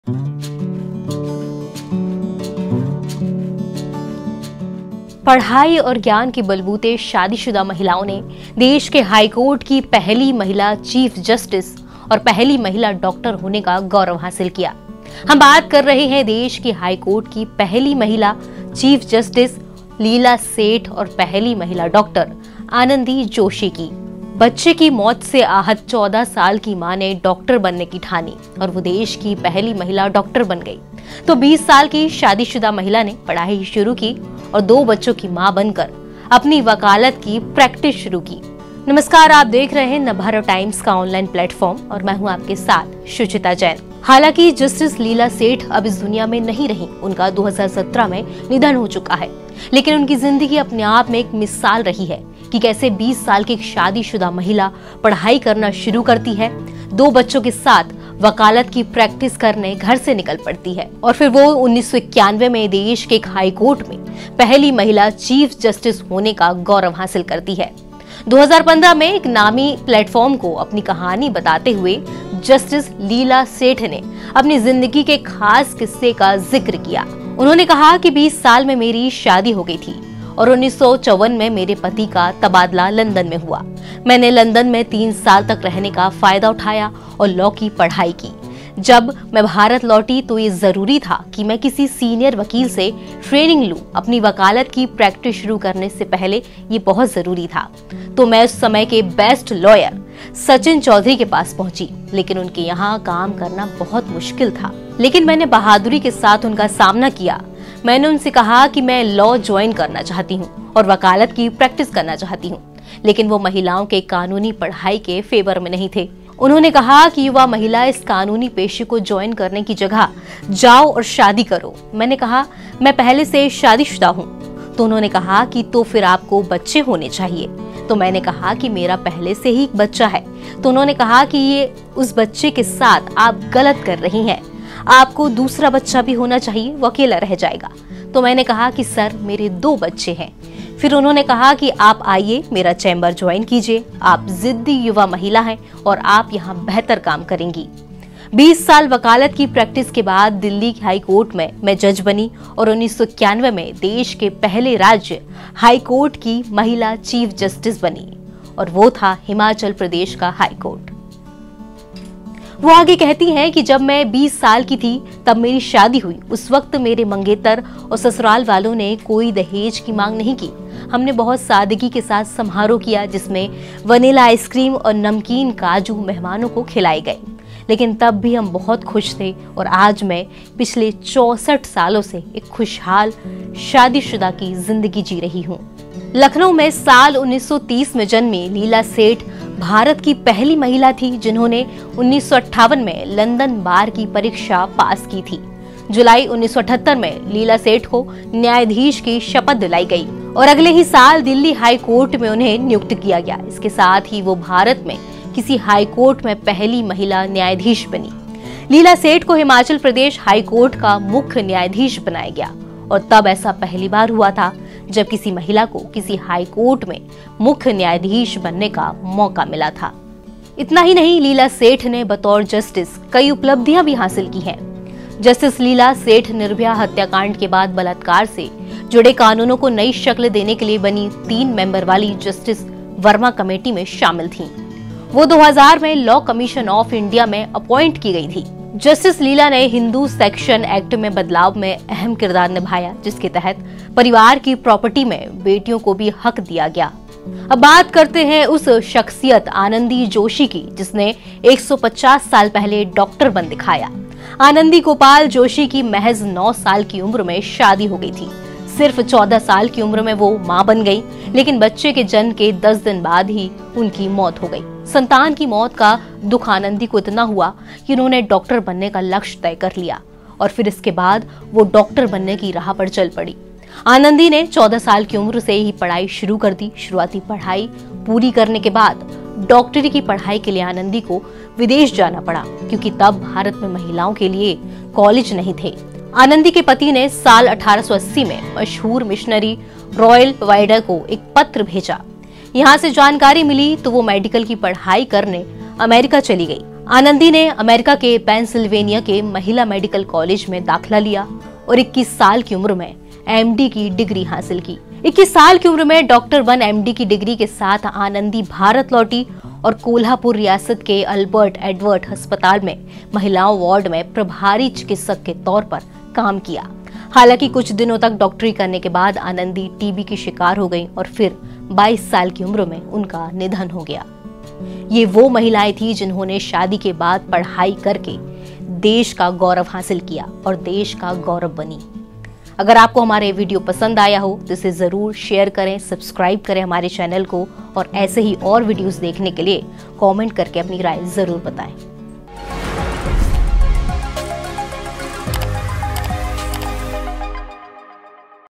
20 साल की उम्र में शादी Leila Seth पढ़ाई कर बनीं देश की पहली State High court Women Chief Justice
20 साल की उम्र में शादी Leila Seth पढ़ाई कर बनीं देश की पहली State High court's Women Chief Justice | Biography of Justice Leila Seth | SDM Jyoti Maurya | Alok Maurya

पढ़ाई और ज्ञान के बलबूते शादीशुदा महिलाओं ने देश के हाईकोर्ट की पहली महिला चीफ जस्टिस और पहली महिला डॉक्टर होने का गौरव हासिल किया. हम बात कर रहे है देश की हाईकोर्ट की पहली महिला चीफ जस्टिस लीला सेठ और पहली महिला डॉक्टर आनंदी जोशी की.